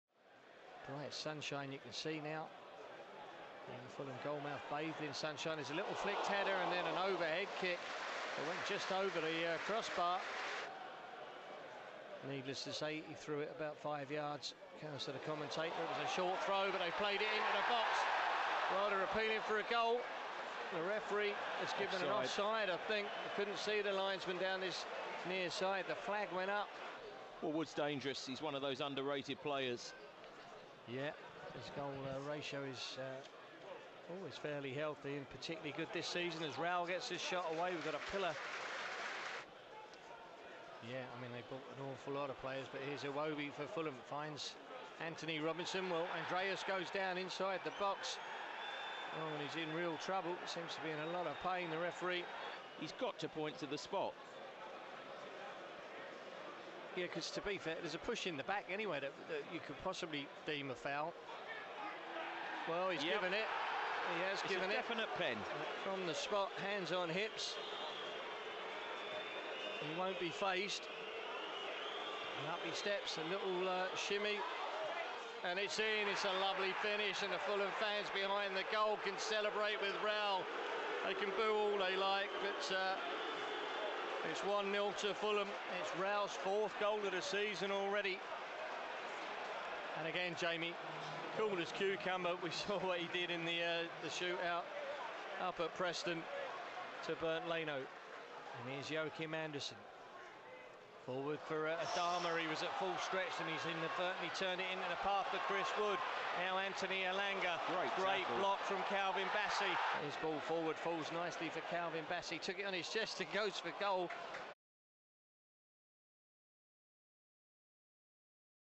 HIGHLIGHTS | Nottingham Forest 0-1 Fulham | Back-To-Back Wins 💪
Watch the key moments as Fulham picked up a solid three points on the road away at Forest.